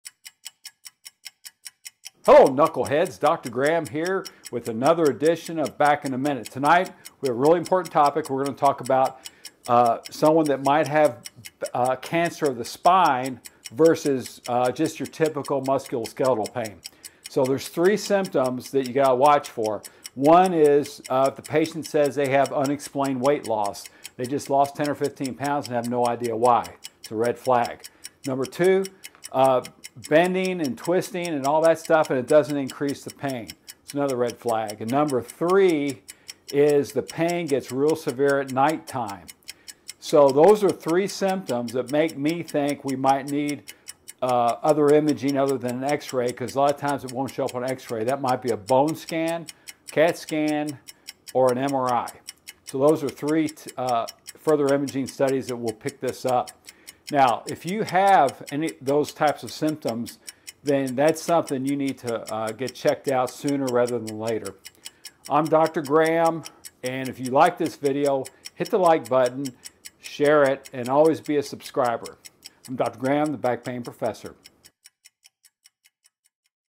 Cancer vs Musculoskeletal Back Pain
Dr. Stephen Graham of Graham Chiropractic in Louisville discusses what are common symptoms of someone who has bone cancer compared to musculoskeletal back pain.
Although the original (primary) cancer is usually diagnosed before back problems develop, back pain may be the first symptom of disease in people with metastatic vertebral tumors.
Top 3 symptoms
unexplained weight loss
pain at night
pain that does not get worse with movement.
Hopefully you do not have these kind of back pain symptoms but if you do you need to get checked sooner more than later!